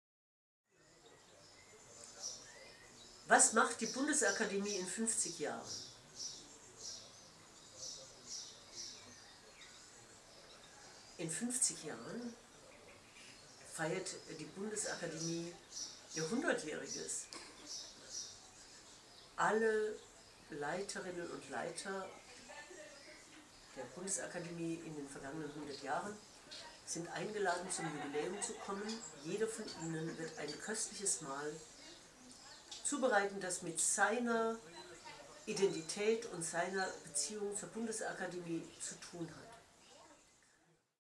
Christel Hartmann-Fritsch: die Bundesakademie in 50 Jahren
Christel Hartmann-Fritsch, geschäftsführendes Vorstandsmitglied der Stiftung Genshagen auf die Frage: die Bundesakademie in 50 Jahren